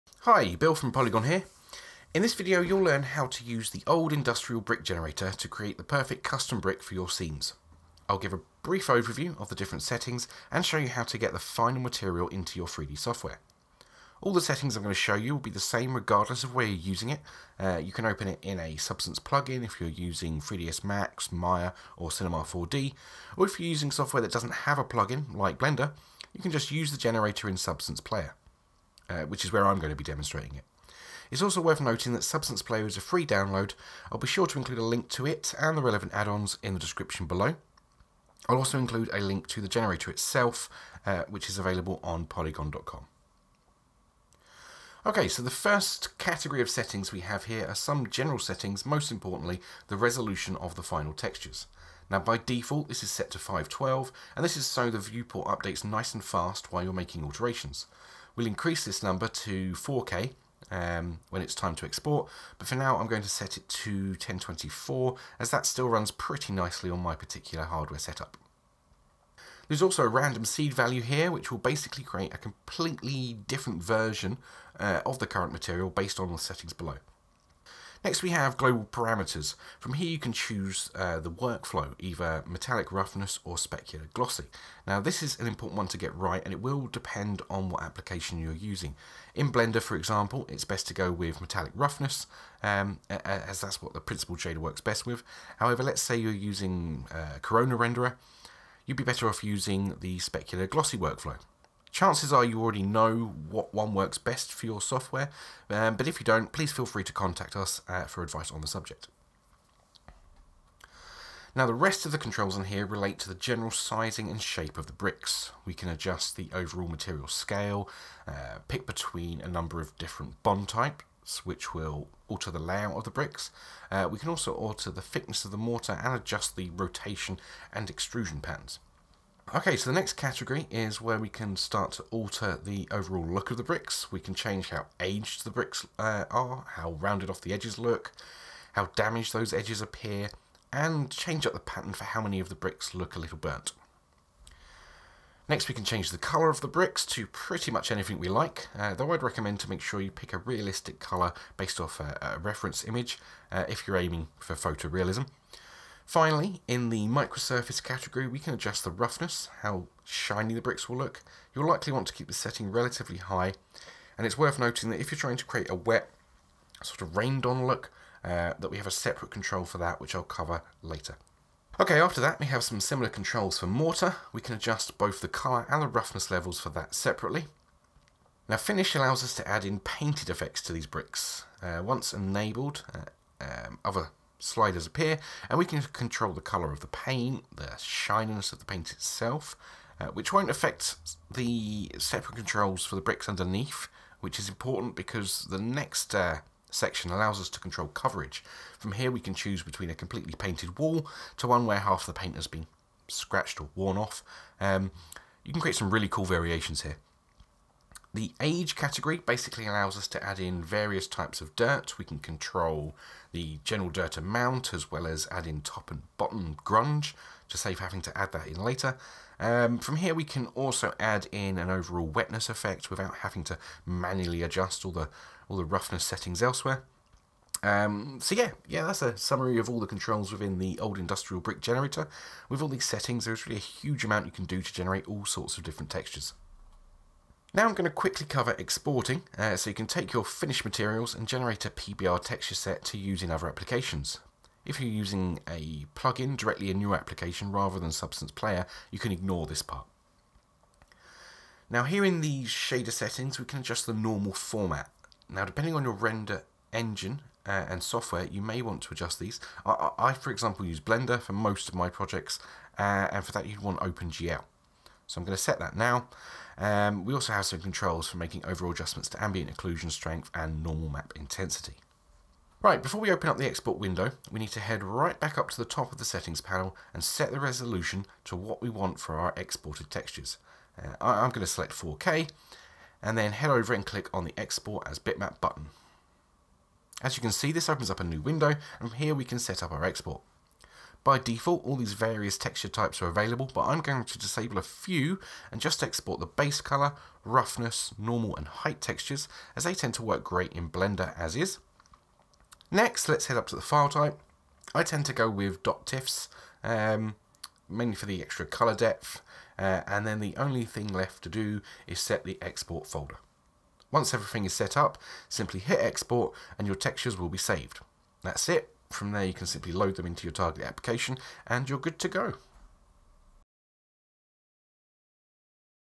How to use the Bricks Old Industrial Generator
In this tutorial, we will take a look at the Bricks Old Industrial Generator now available on Poliigon. SBSAR files are Substance Designer archives that allow us to provide you with easy to customize materials that can be used in almost any 3D software.

For more information on how to use SBSAR files please visit our help article on the subject: